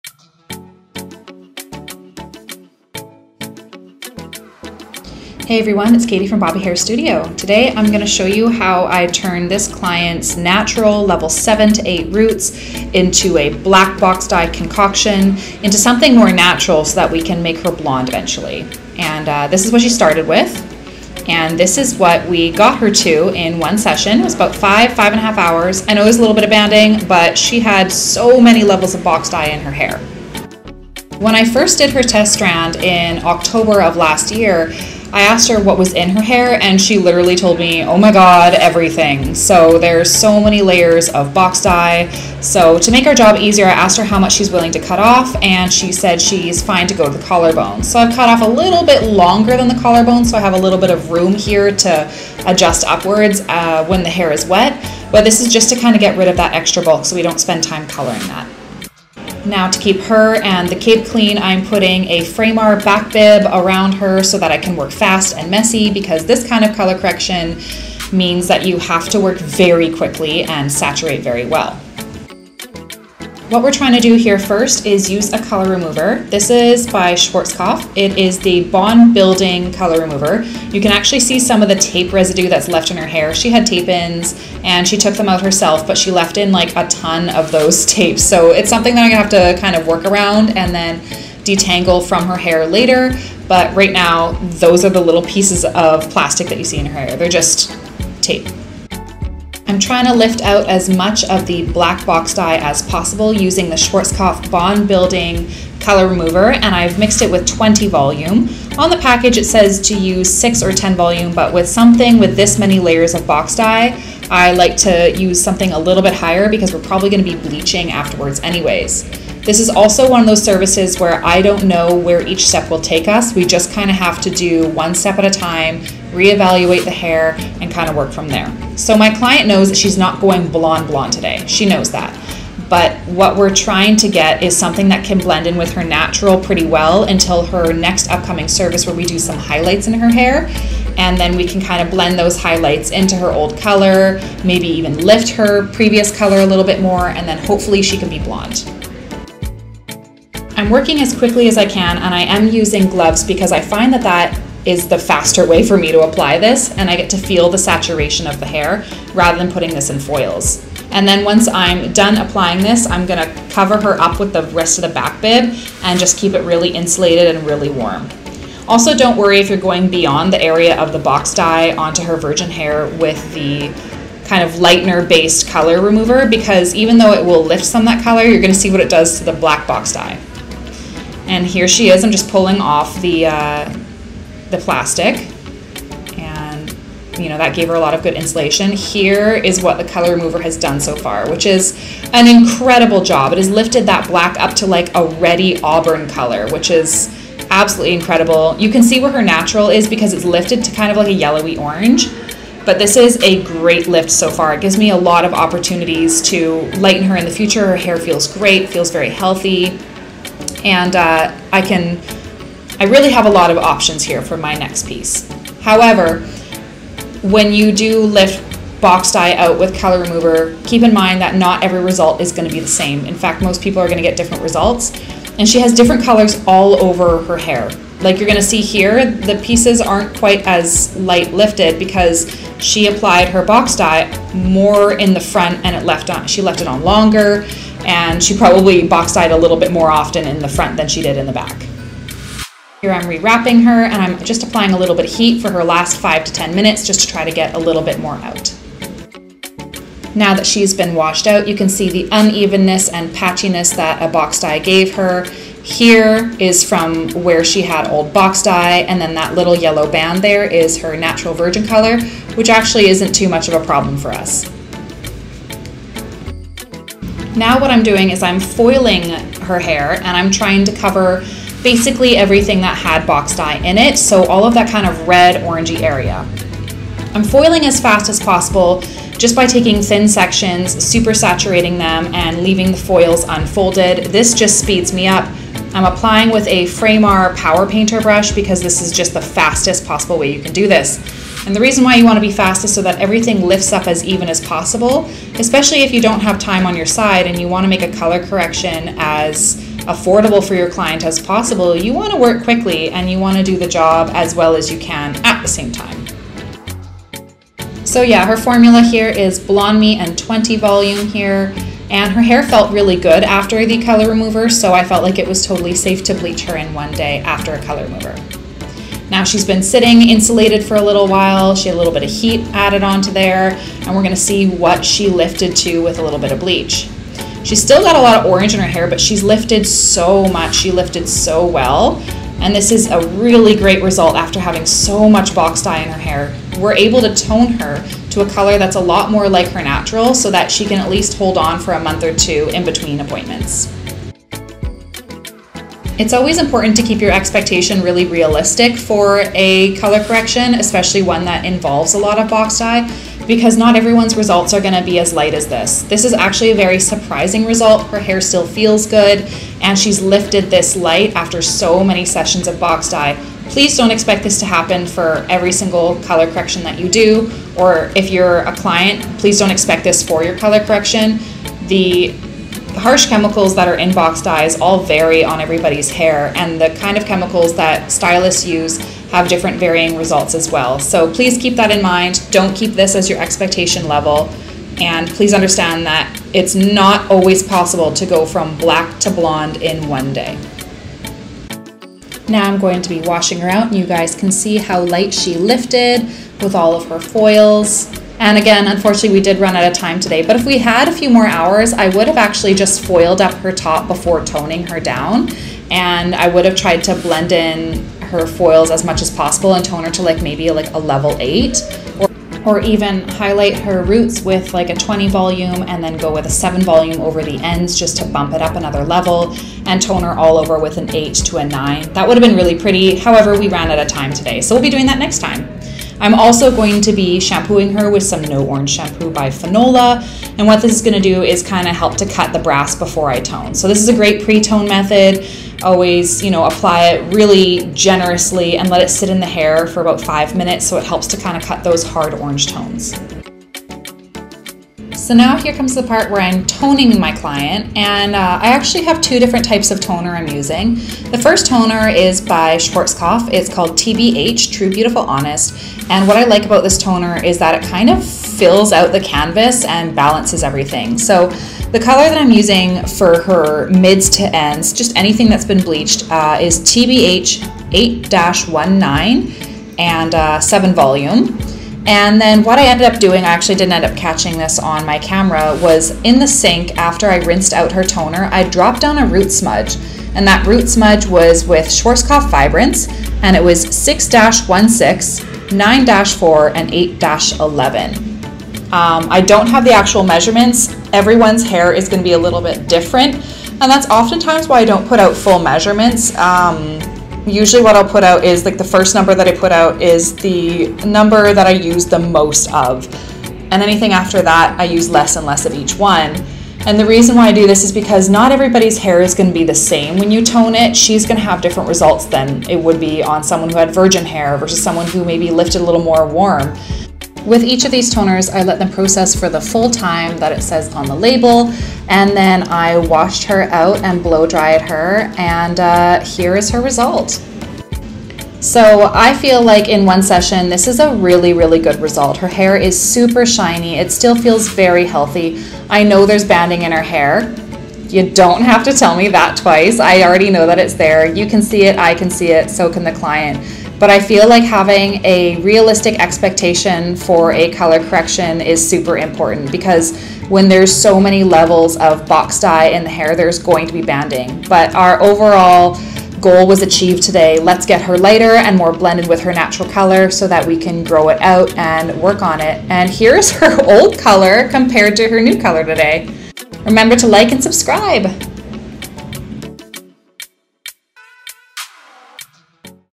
How to remove black box dye part 1 - hair color transformation before and after
Hey Everyone! Todays video is a quick tutorial on how I remove black box dye from the hair. I often times like to use this method as its more gentle on the hair. My plan is to have this client back in a month to do her blonde foils, but this was great so that we can have a nice blend between the foils and not have any black colour left! let me know what you think of this tutorial below!

PART 2 IS NOW AVAILABLE!!! GO CHECK IT OUT, BABES!